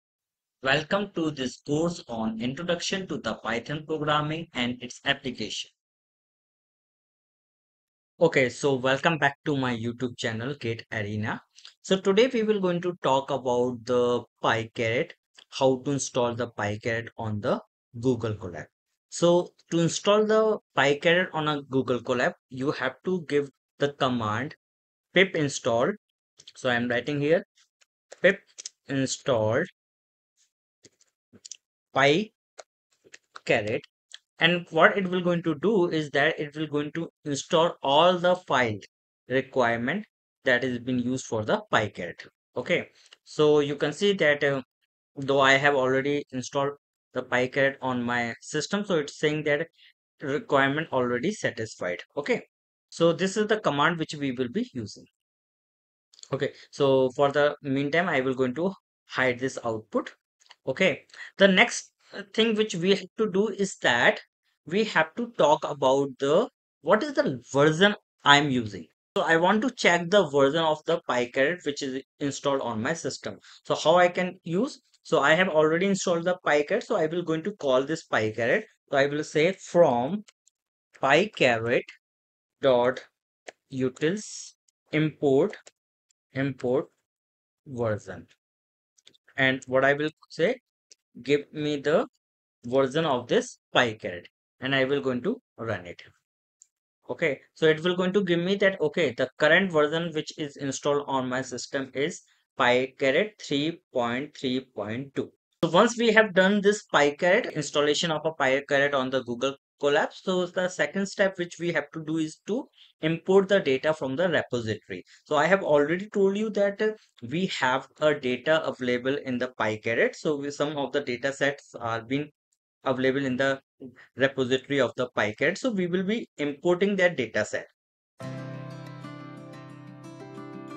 PyCaret Tutorial: Installing PyCaret on Google Colab
Today, we are going to dive into the exciting world of machine learning with PyCaret on Google Colab. If you're looking to streamline your machine learning workflow and build powerful models without the hassle of traditional coding, then PyCaret is the tool for you. In this tutorial, we'll show you how to get started with PyCaret on Google Colab, a free cloud-based platform that allows you to run Python code right in your browser. So, grab your notebooks and let's get started!

[Setting up Google Colab]
First things first, let's set up Google Colab for our PyCaret journey. Simply open your browser and go to colab.research.google.com. If you don't already have a Google account, you'll need to create one to access Colab. Once you're in, click on "New Notebook" to create a new Python notebook.

[Installing PyCaret]
Next, we need to install PyCaret on Google Colab. To do this, simply run the following command in a code cell:

!pip install pycaret

PyCaret will now be installed on your Google Colab environment, making it easy to access all the powerful machine learning tools it has to offer.

So why wait? Start your machine learning journey with PyCaret today and unlock the full potential of your data. Happy coding!